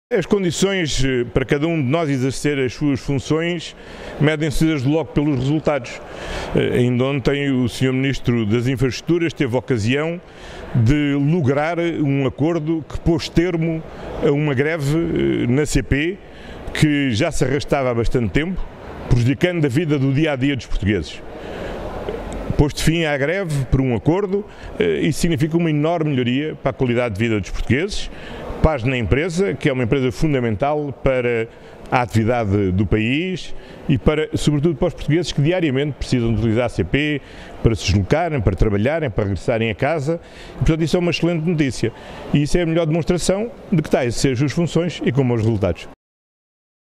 Costa diz que futuro político do ministro João Galamba depende dos resultados
O primeiro-ministro recusou hoje que a manutenção do ministro das Infraestruturas seja um fator de problemas políticos para o Governo, contrapondo que o futuro de João Galamba depende dos resultados que alcançar no exercício das suas funções.